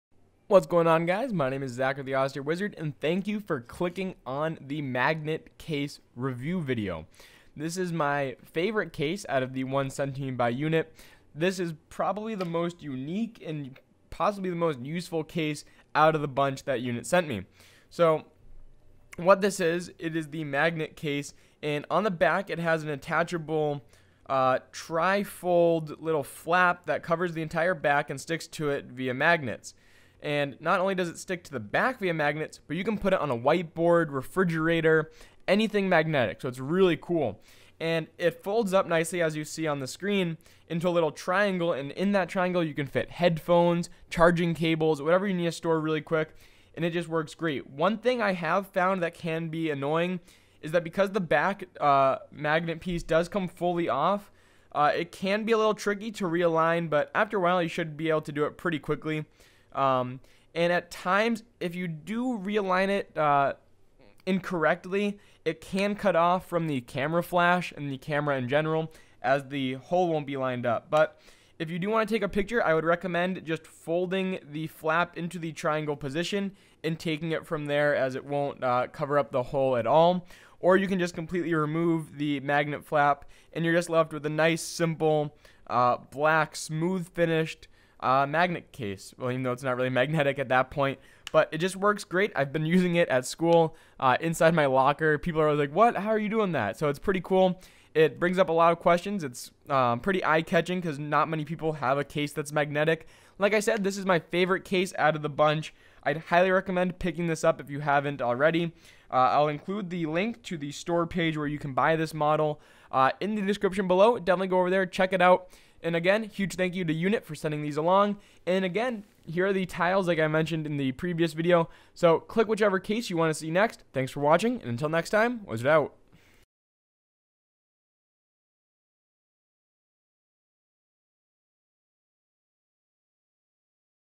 UNIT Magnet Case for iPhone 5/5s
This is definitely my favorite and a one of a kind iPhone case! The Magnet Case for iPhone 5/5s by UNIT is great! Offers solid full back protection and has the addition of the magnetic backing. The magnetic back can be folded into a triangle and be used to hold your earbuds, charger cabler or any sort of cable. You can stick it to your fridge, locker at school, whiteboard or ANY magnetic surface. You'll get a lot of great reactions when you stick your phone to something. Don't worry, the magnets won't mess up the inner workings of your phone and everything will function as normal. The magnetic back will wear down a bit after heavy use but it still remains fully functional.